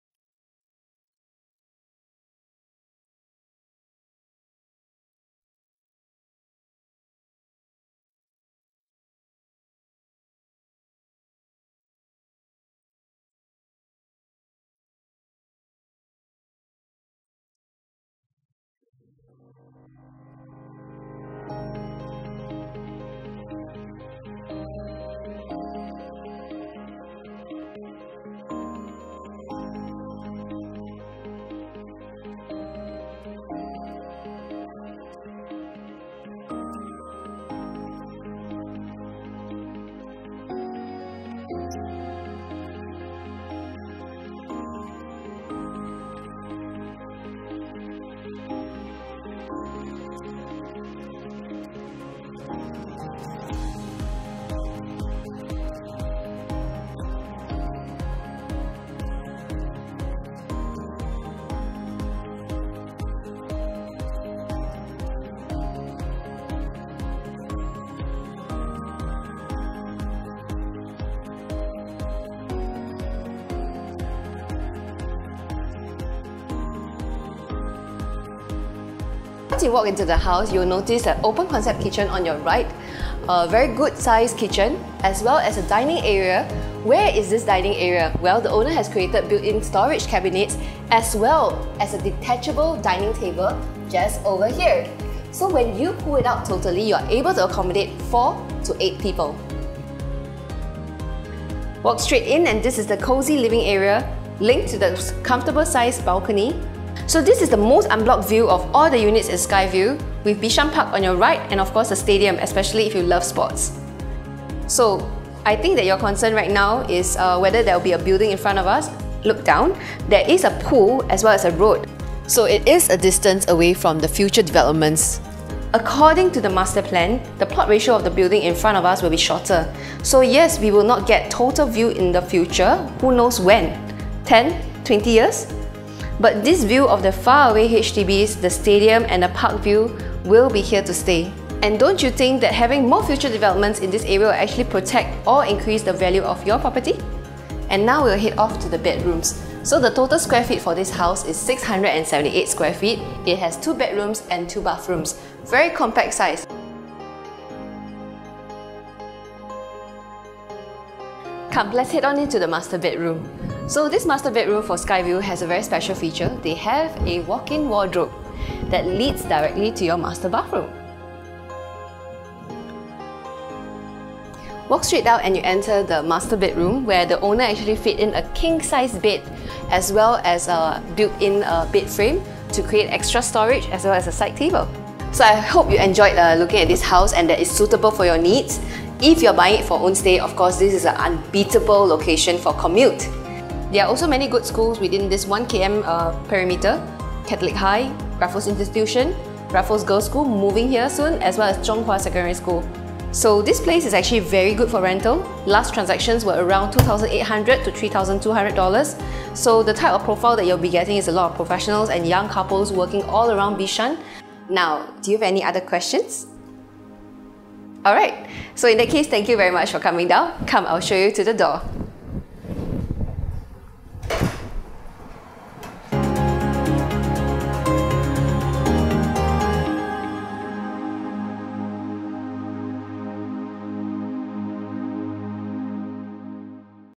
[SOLD] Sky Vue: 2 Bedroom Condo located in Bishan Central With A Panoramic View (S$1.19M)
[SOLD] Sky Vue: 2 Bedroom Condo located close to Bishan MRT

Time Stamps:
00:00 Start of Video
00:13 Amenities
00:37 Facilities
01:02 Entrance
01:21 Kitchen and Dining Area
01:40 Living Area and balcony
02:27 Common Bedroom and bathroom
02:42 Master Bedroom and bathroom

Panoramic, unblocked view of the landed estate from the serenity of your balcony
✅ Main door faces East; Living room faces West
✅ Living room is cleverly renovated with built in cabinets for extra storage
✅ Cozy living area which extends out to the balcony for an airy and bright space for the family to gather around in
✅ Open concept kitchen equipped with  branded kitchen appliances
✅ Both bedrooms are able to enjoy full privacy while looking out to an unblocked view of Bishan and beyond.
✅ Both bedrooms are squarish in layout and has no bay windows (no wastage of space!) - able to fit  a queen size bed comfortably
✅ Amazing walk-in wardrobe in master bedroom
✅ Perfect for your own stay or as an investment property!
✅ Average Rental for 2 Bedrooms - $2800 to $3000, approx 2.7% Rental Yield.
✅ Total 2 blocks with 694 units in this condo
✅ Enjoy the facilities: Includes a Clubhouse, Function room, 50m Lap pool, Sun deck, Aqua gym, Yoga deck, BBQ area, Children's playground, Indoor gym, Tennis court, Outdoor fitness station, Sky lounge & Sky gym @ level 37 and Karaoke lounge

Amenities:
Great location with many amenities!
✅ 6 Mins walk to Bishan MRT & Bus Interchange
✅ 7 Mins to Kim San Leng Food Centre & Giant Supermarket @ Bishan Blk 512
✅ Minutes walk to Junction 8 Shopping Mall & Bishan Swimming Complex,  Bishan ActiveSG Stadium, Bishan Community Club
✅ Nearby schools: Raffles Institution, Kuo Chuan Presbyterian Primary School, Kuo Chuan Presbyterian Secondary School, Townsville Primary School, Guangyang Secondary School, Catholic High School and many more.

Friendly note: timings may differ depending on traffic conditions.

Contact Diana Teow @ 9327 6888 for exclusive viewing!